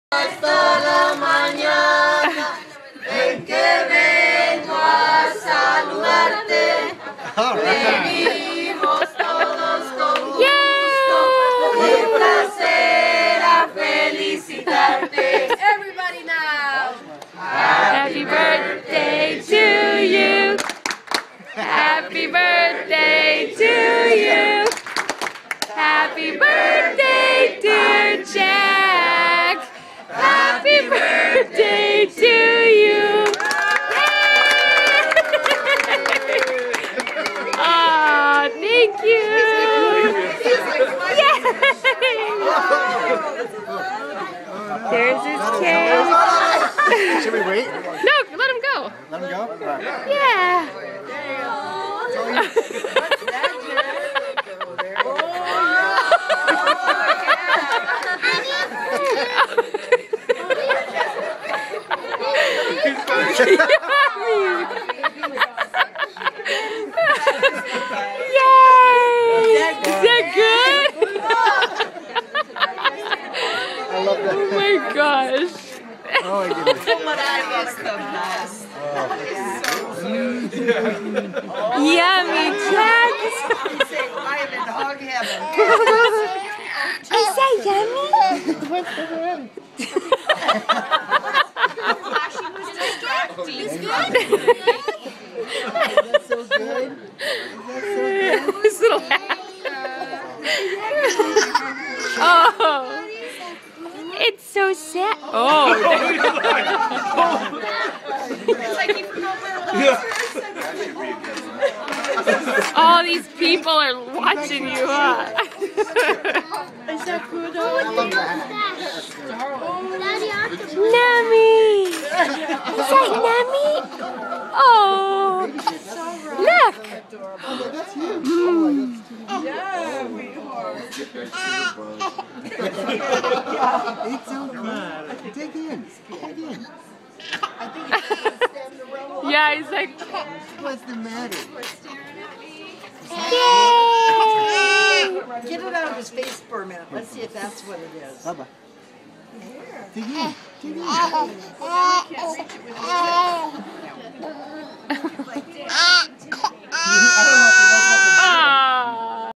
Jacks 1st birthday
Nothing like your first birthday at Aztecas! Haha